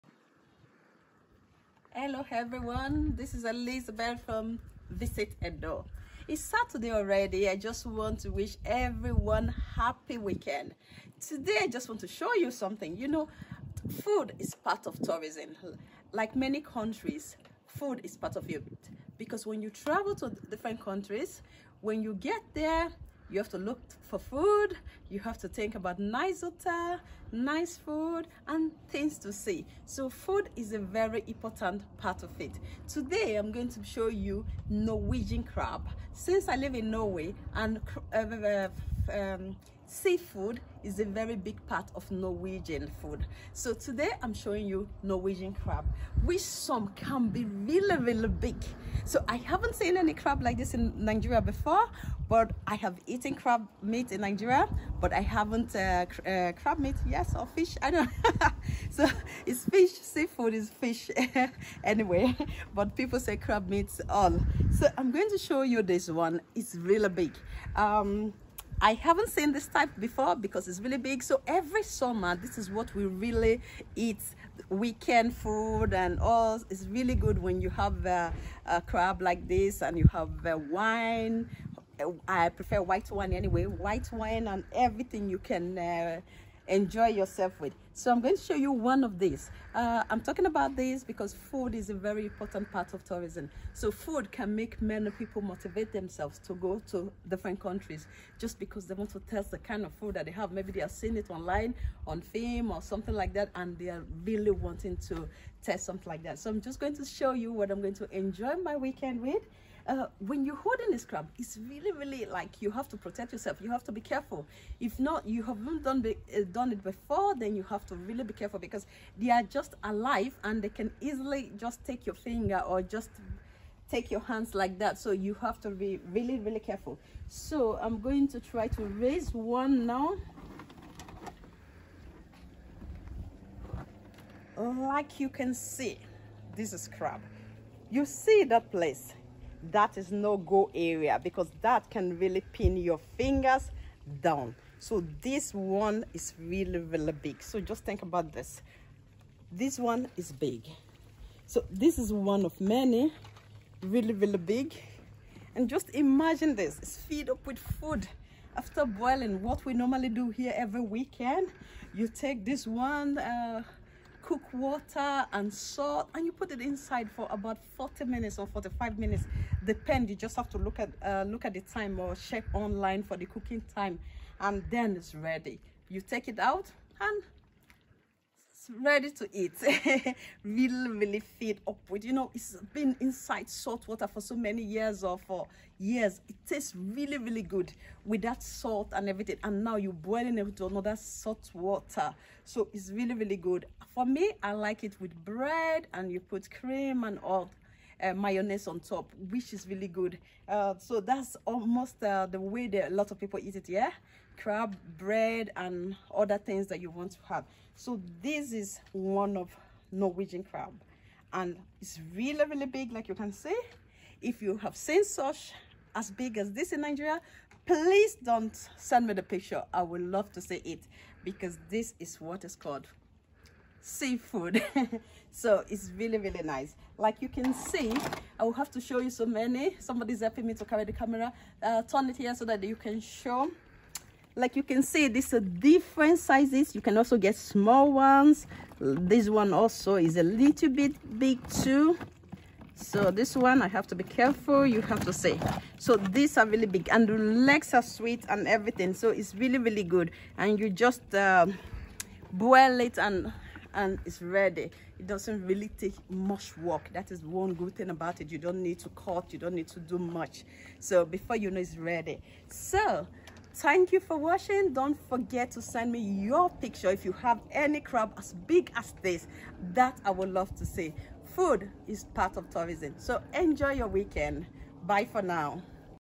Greetings from VisitEdo team . Please enjoy your weekend with this video of sea food 😁❤️
Greetings from VisitEdo team . Please  enjoy your weekend with this video of sea food 😁❤️ Food is a very big part of tourism and I believe Edo has what it takes when it come to tourism…